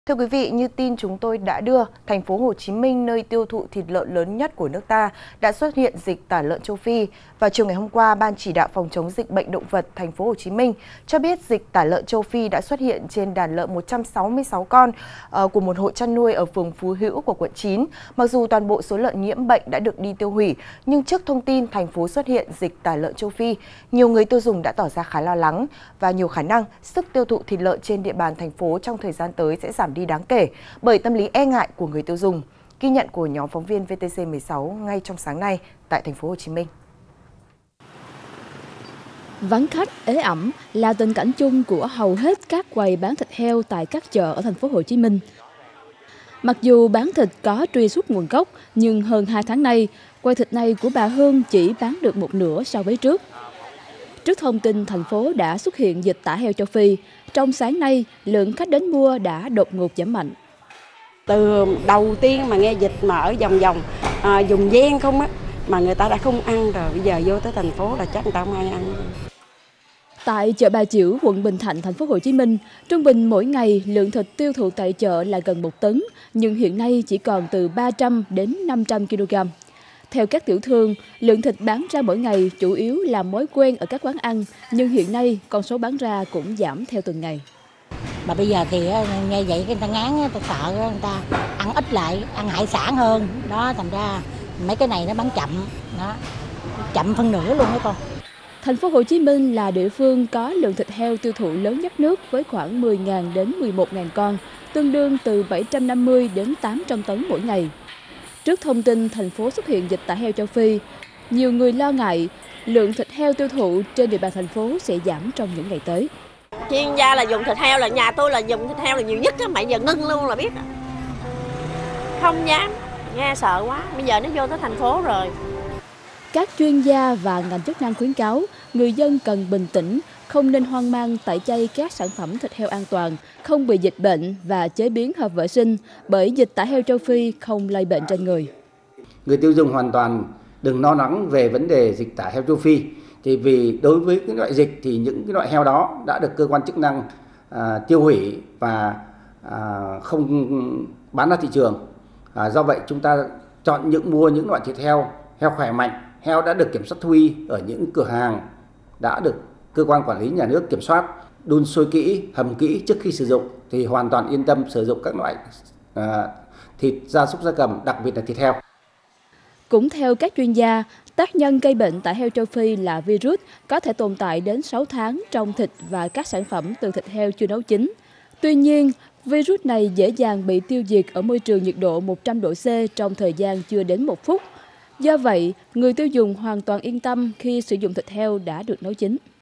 TP.HCM: Thịt lợn ế ẩm do dịch tả Châu Phi | VTC16
VTC16 | Thành phố Hồ Chí Minh, nơi tiêu thụ thịt lợn lớn nhất nước đã xuất hiện dịch tả heo châu Phi. Nhiều khả năng, sức tiêu thụ thịt lợn tại đây sẽ giảm đi đáng kể bởi tâm lý e ngại của người tiêu dùng.